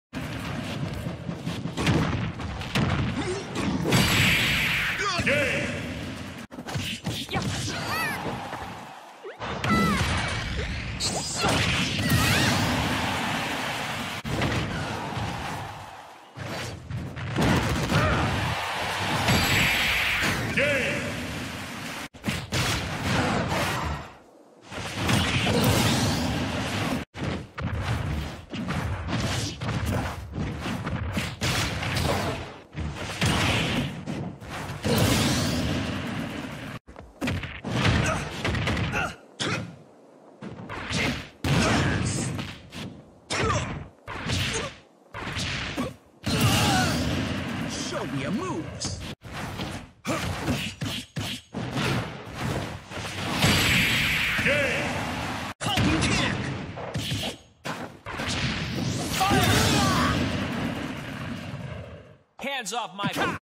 February Hype Reel
Shorter this month because I was also focusing on the cring compilation and something else I might post before fall

These were in version 10

Mythra is busted gonna start playing her a lot expect some clips featuring her for a while.